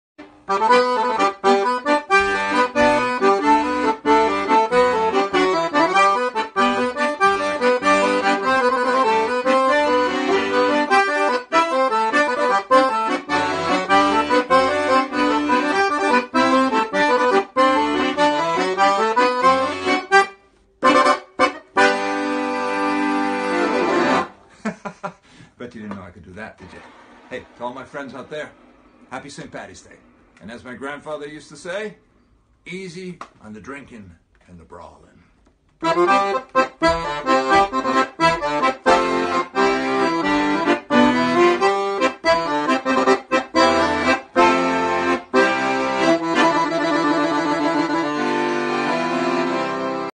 Thomas Ian Griffith playing the Accordion for St. Pat's 2018- Upscaled by me from TIG's Twitter page
🪗 "A little toast before you head out for the night... HappyStPatricksDay!"🍀 - Thomas Ian Griffith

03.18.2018

My first attempt at upscaling such a tiny 192×380 mobile video file, sorry for the wobbly lines! 💖